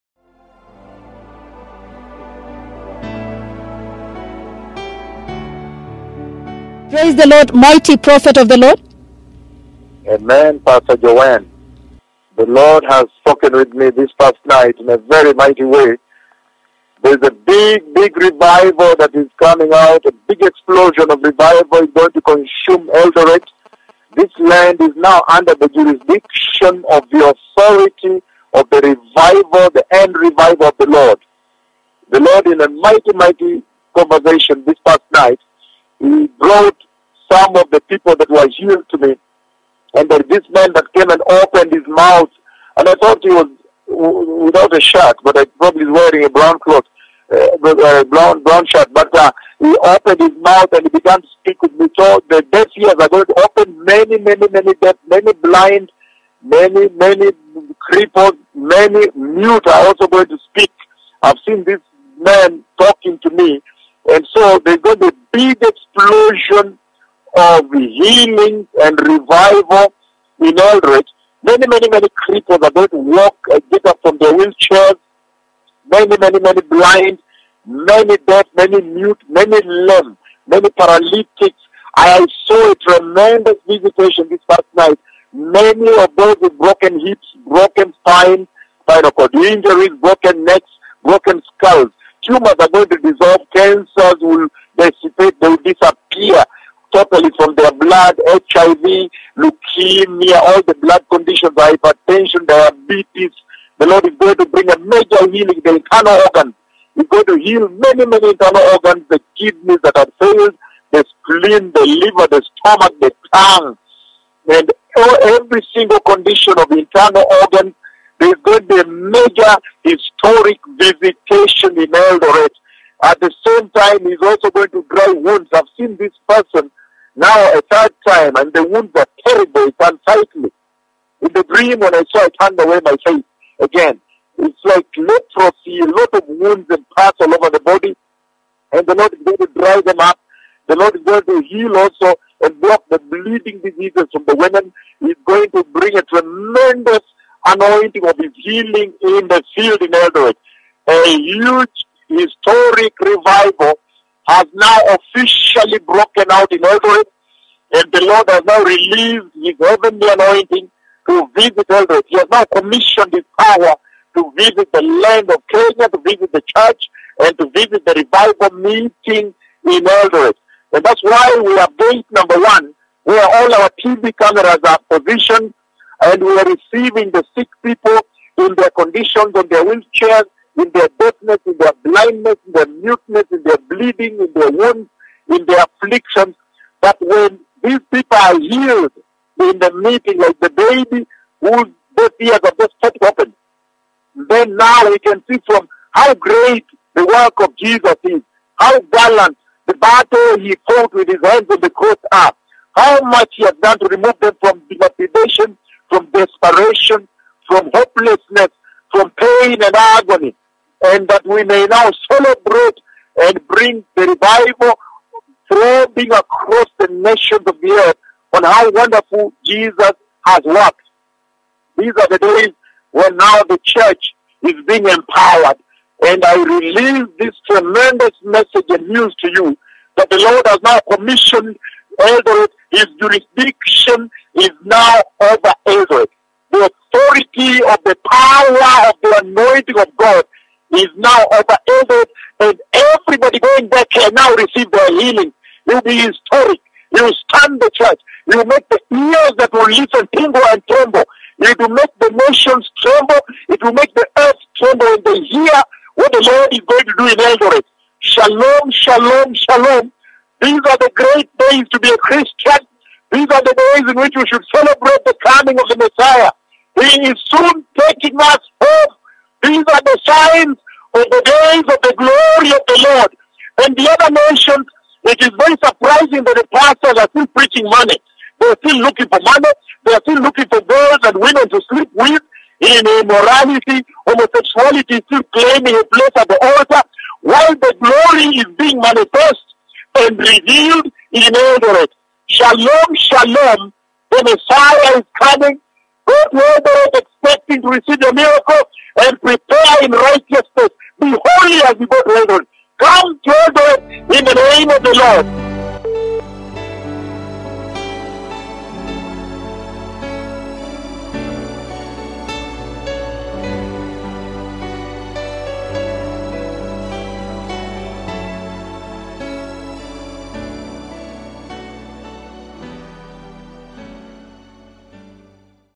7th PROPHECY! BIG EXPLOSION OF HEALING & REVIVAL COMING TO THE ELDORET MEETING - PROPHET DR. OWUOR
PROPHECY OF A BIG EXPLOSION OF HEALING & REVIVAL COMING TO THE ELDORET MEETING - PROPHET DR. OWUOR  Live August 27th, 2015!  Mobilizing the global church of Christ for the glorious End Time Revival, to prepare the Way for the Coming of the Messiah!  Let us prepare in righteousness, and holiness for the wedding supper of the Lamb is nigh!!

Shalom!